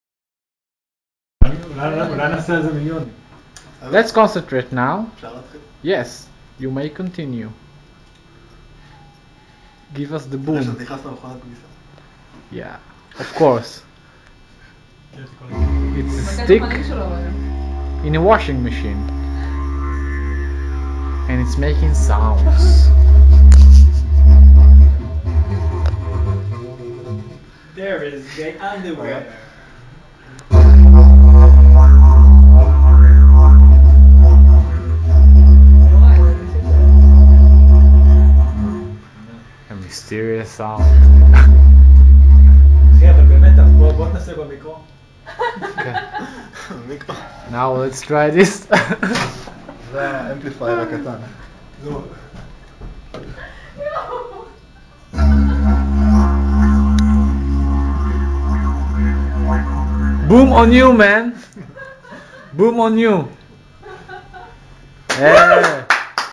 Attempt To Amplyfi My Didge
me and my friends try to play the didge in the washing machine in order to get cooler sound :)